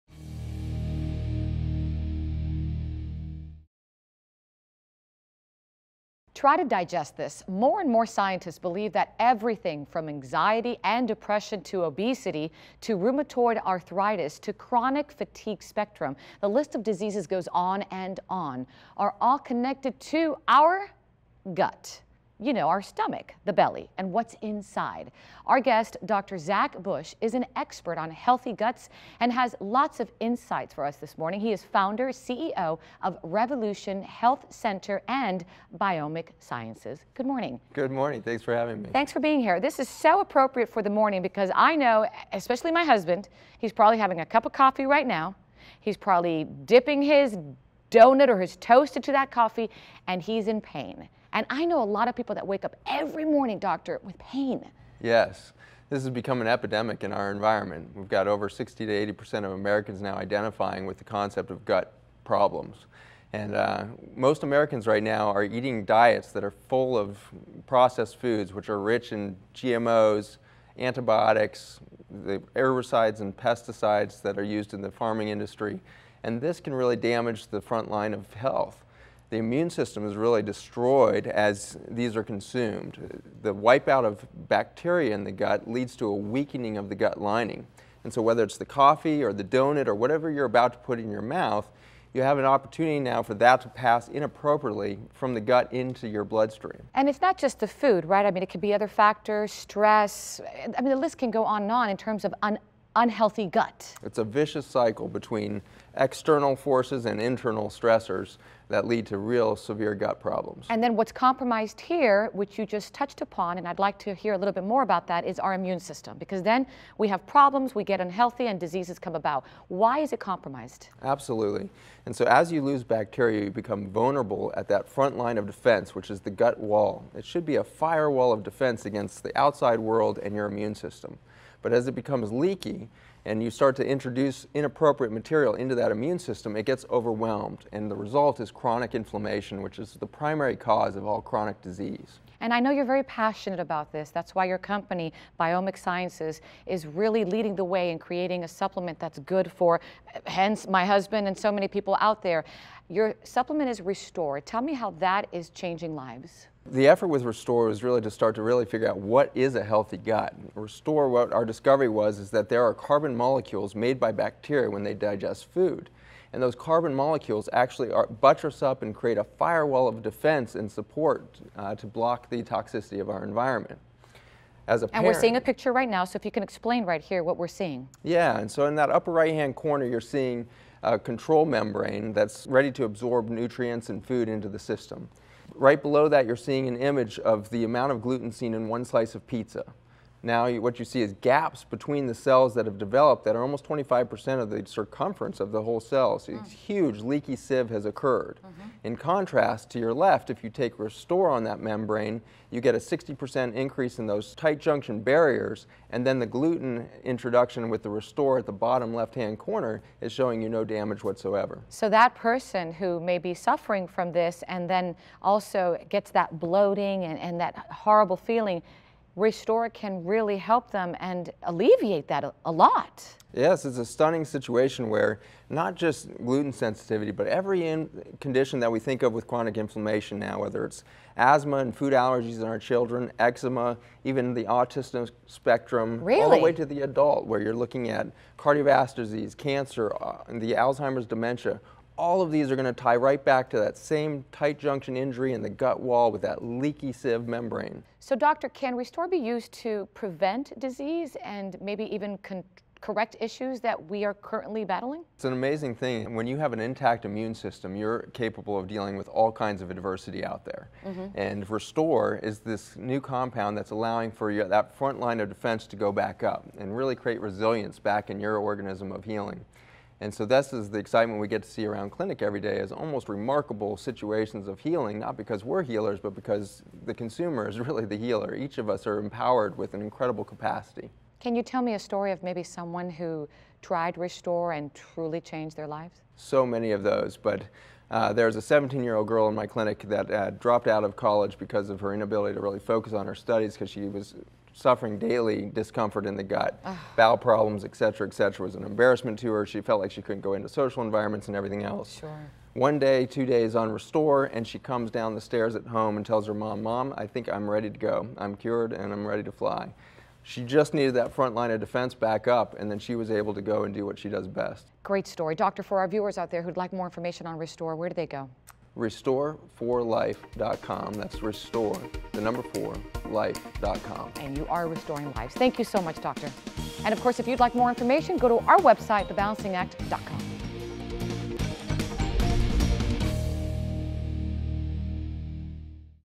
What Does Your Gut Say About Your Overall Health? - The Balancing Act
More research points to a link between what’s going on in our belly.. and the diseases that we’re battling. Seventy-percent of our immune system is found in our gut.  And a bacterial imbalance in the gut can lead to elevated inflammation that can advance disease – such as inflammatory conditions in our children and adults:  food sensitivities, allergies, asthma, eczema, hormone imbalances, attention deficit disorder, autism, vascular disease, cancer and dementia. The dietary supplement – RESTORE can help.  RESTORE supplies the first line of defense against gut dysfunction.  It is our gut’s firewall of protection against infection, damage from environmental and food-borne toxins. Remember: good gut leads to good health. You don’t want to miss this!

102750